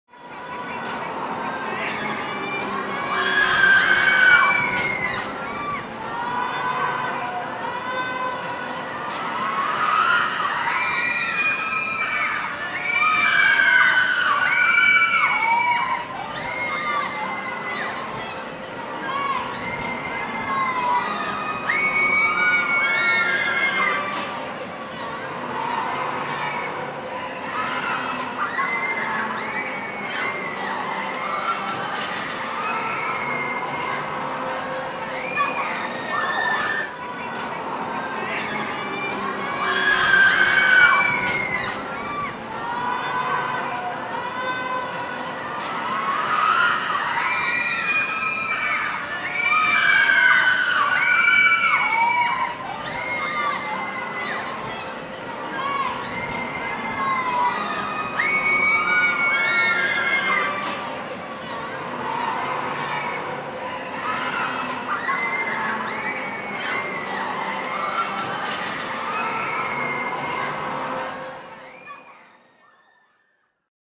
Texas Sky Screamer Animation
In 2013, prepare to soar to new heights on the all-new Texas SkyScreamer at Six Flags Over Texas.

Towering an astounding 400-feet above the ground, the Texas SkyScreamer will be the tallest swing ride in the world that only looks sweet and innocent. Once you are strapped into the open-air seat of this extreme swing ride, prepare for takeoff. The Texas SkyScreamer will start to slowly swing you around and around as it takes you all the way up to the very top of the tower where you'll spin around and around just below a gigantic Texas Lone Star. Before you know it you'll be going faster and faster, soaring in a 124-foot-wide circle with the wind blowing in your face at 35 mile per hour and your feet dangling in sheer terror 400-feet above Six Flags Over Texas.